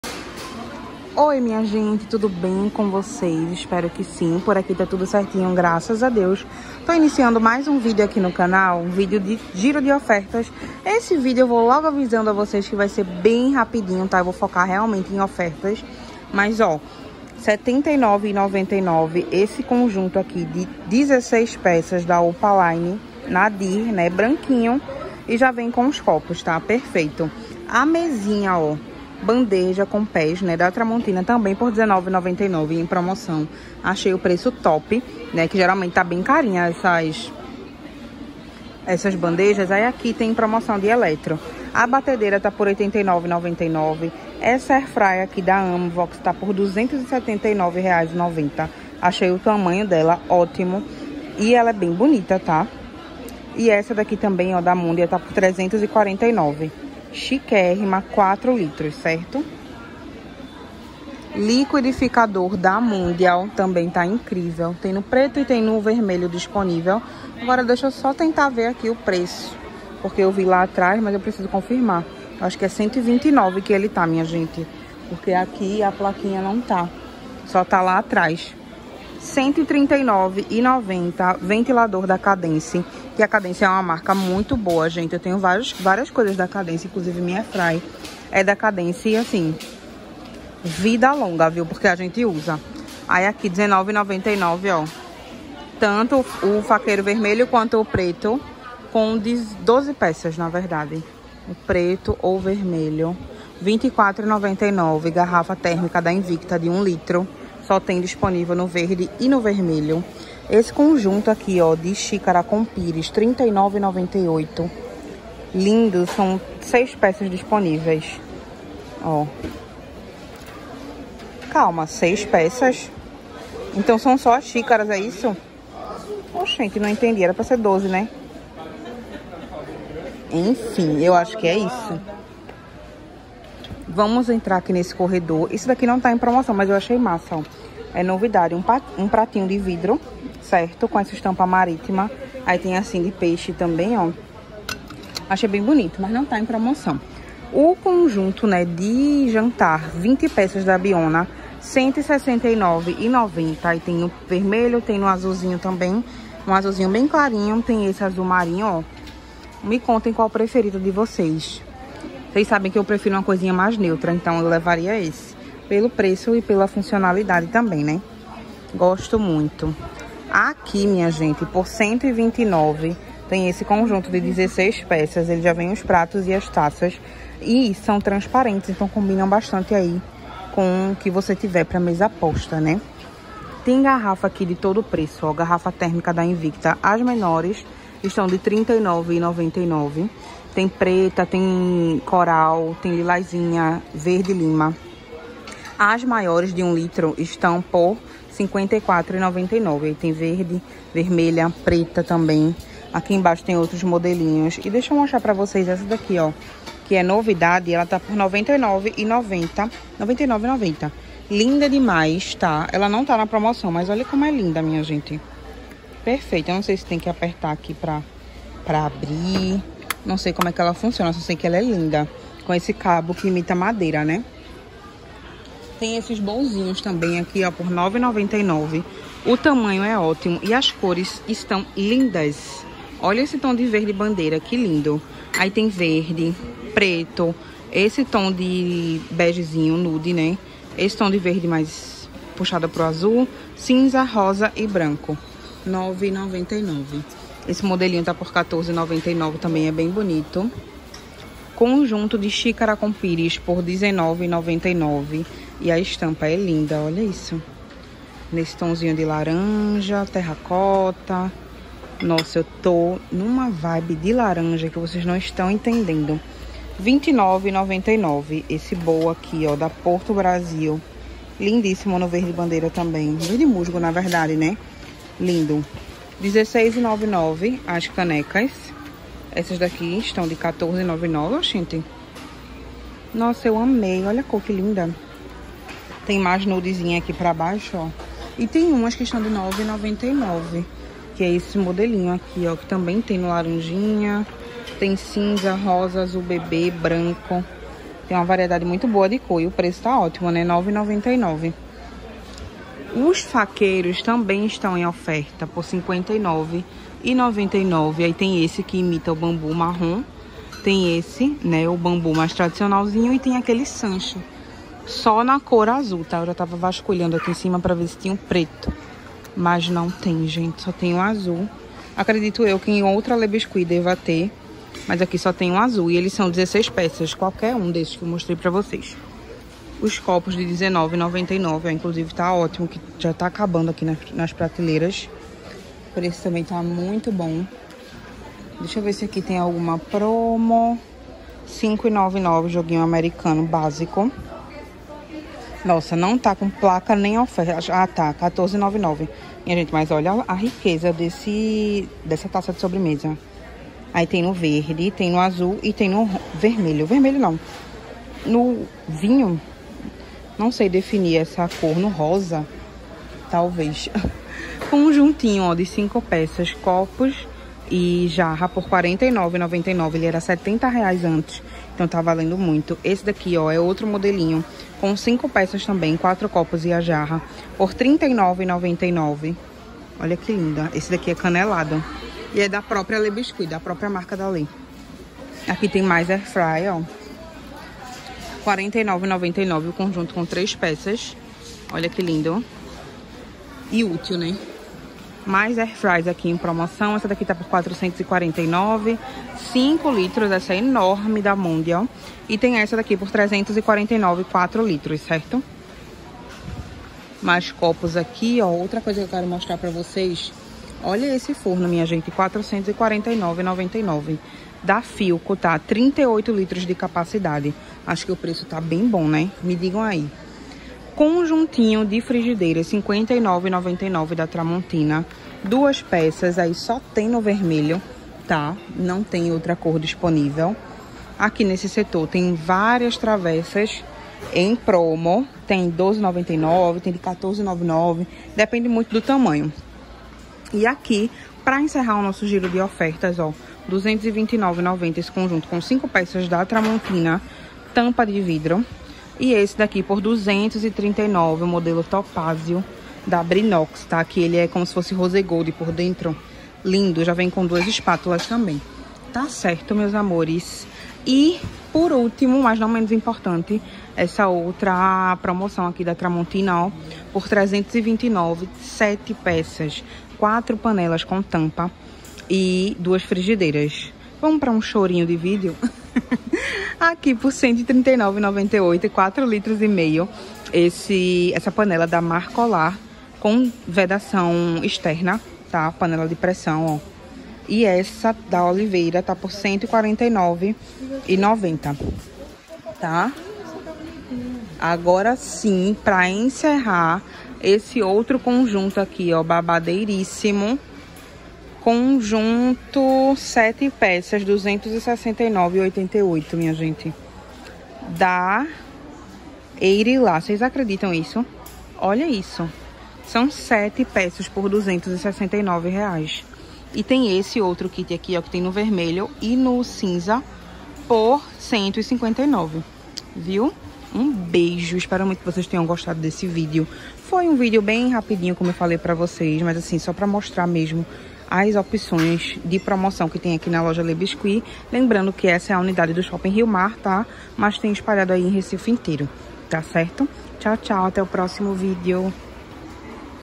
A LÊ BISCUIT está cheia de promoções e novidades 🤗😍 Pirei nos preços baixos 🛍💕
Oiiiii minha gente mais um vídeo de achadinhos, sei que vocês amam e eu também!

Endereço- loja LêBiscuit Riomar shopping